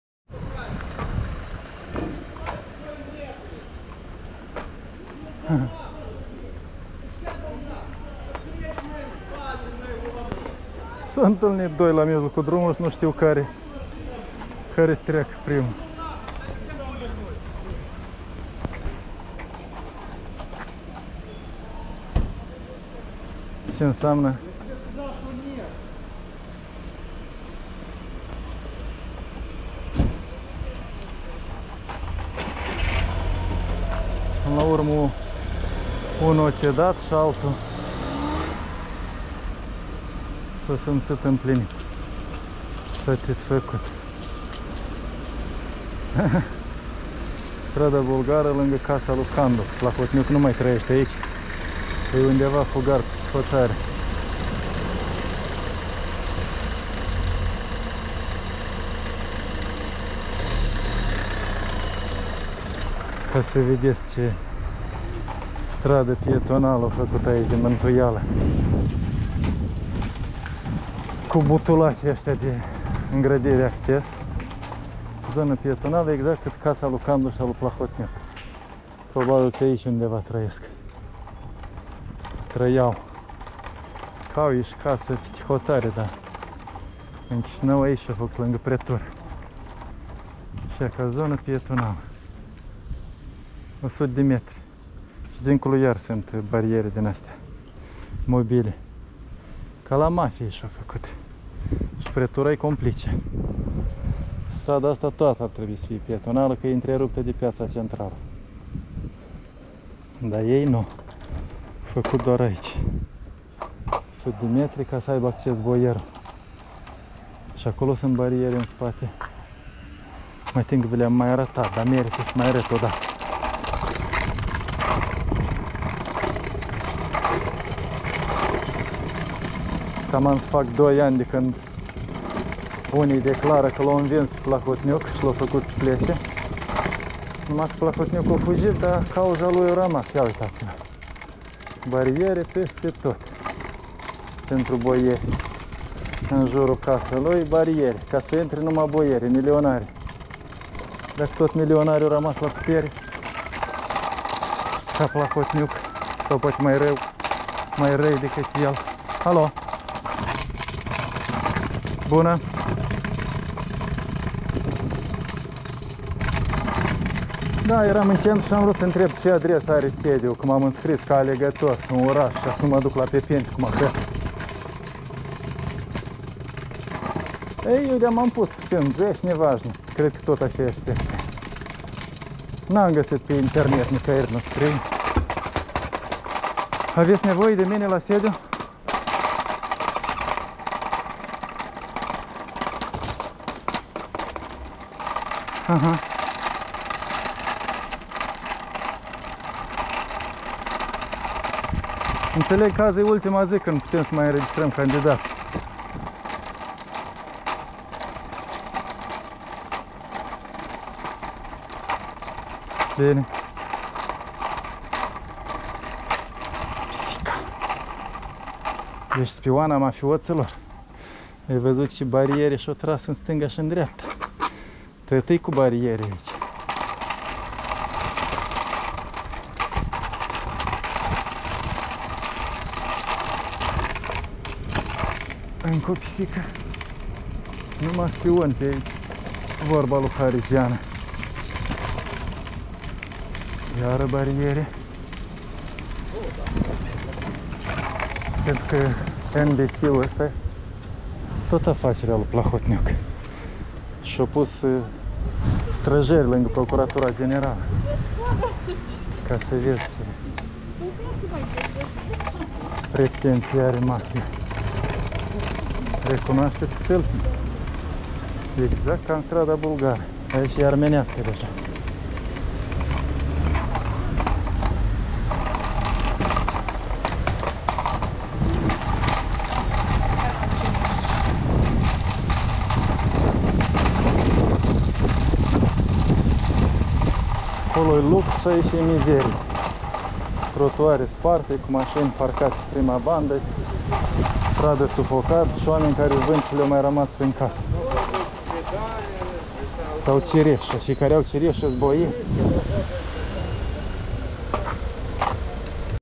Şoferi încăpăţînaţi şi bariere în jurul casei lui Candu/Plaha - Curaj.TV
Ceartă surprinsă la final pe strada Bulgară, mai la deal de Pretura Centru, chiar la colţul casei unde locuiesc Candu şi nănaşul său-fugar, Plahotniuc. Acolo e plin de bariere, la doi ani după căderea regimului mafiot. Se pare că cineva vrea să-l moştenească